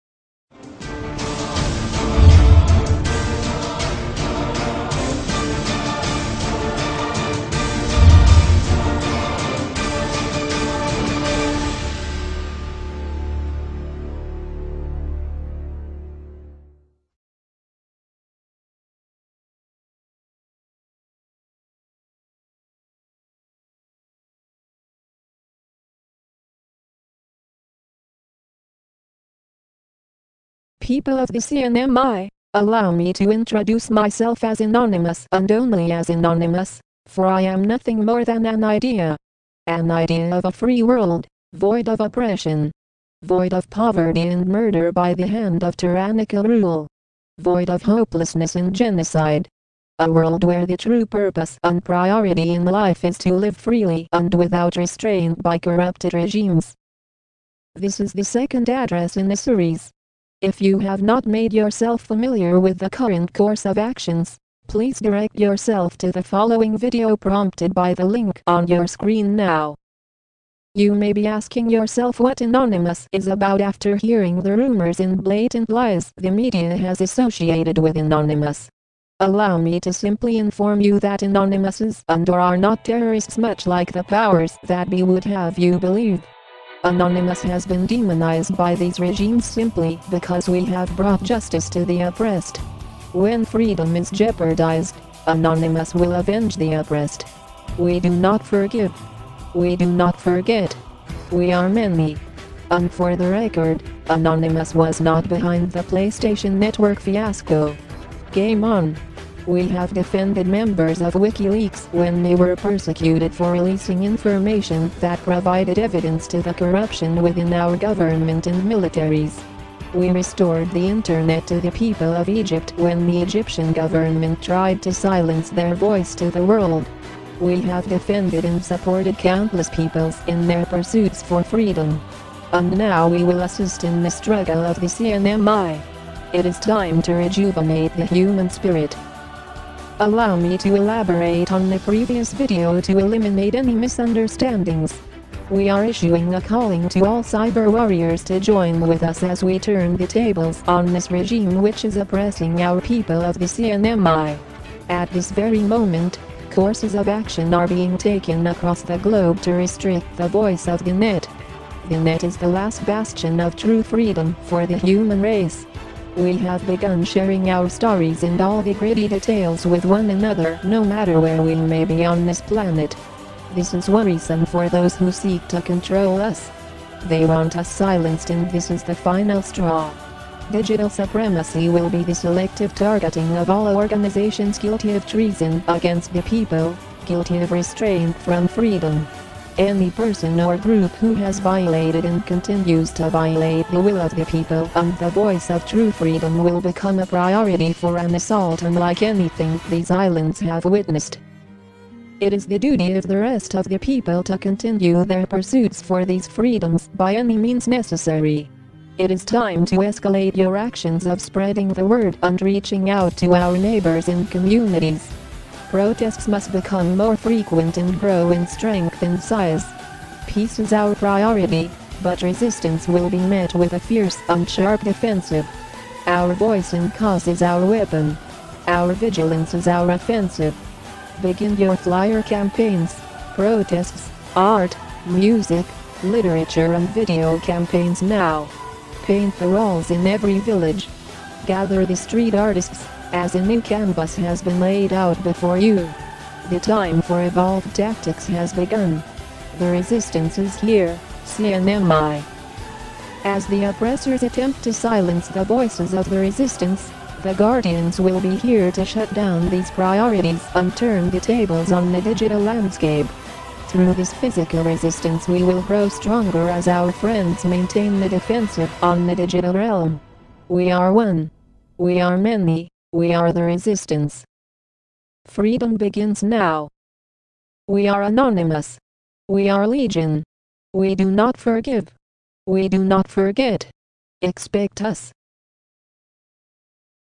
Anonymous Saipan - Resistance Begins NOW!
Greetings, people of the CNMI.  The long-awaited Resistance has arrived in our Islands.  We are the people.  We are Anonymous.  You are Anonymous.  No longer will we stand by and watch our country, our islands, our HOME, continue to degrade.  The Resistance is here.  The Resistance begins NOW.  Speak out against the corruption of the government!  Speak out against the corruption of the police force!  Speak out for those who are still afraid to do so!  We are Anonymous.  Join us.

Expect us.